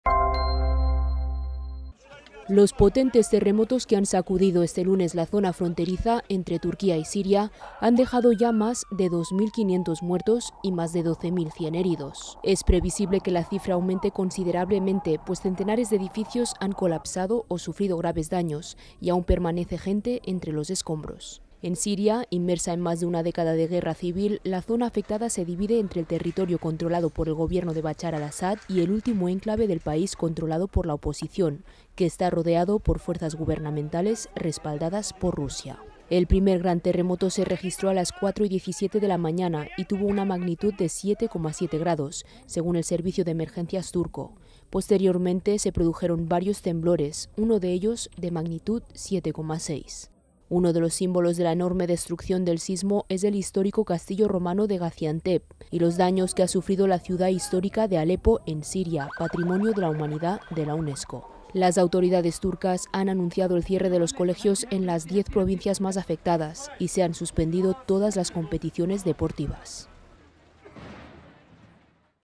Más de 2.500 muertos por el sismo: al menos 1.541 en Turquía y 968 en Siria
Turquía / Siria, 6 feb (EFE/EPA).- La serie de potentes terremotos que ha sacudido hoy la zona fronteriza entre Turquía y Siria ha dejado de momento 2.509 muertos y más de 12.100 heridos en los dos países, y ha derruido miles de edificios.
INCLUYE IMÁGENES CEDIDAS. SOLO USO EDITORIAL.